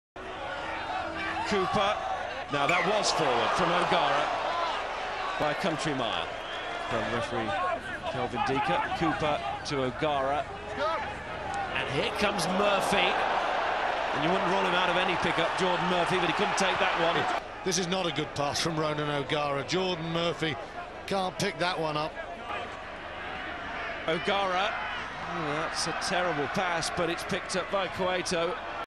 Ronan O'Gara sloppy passing vs Southland 2005
British & Irish Lions fly half Ronan O'Gara's passing against Southland leaves a lot to be desired.

0:00 - O'Gara throws a forward pass by "a country mile".
0:07 - O'Gara throws a poor pass right on Geordan Murphy's toes who then knocks on.
0:22 - O'Gara throws a "terrible pass" that goes to nobody but luckily for him it bounces up okay for Mark Cueto.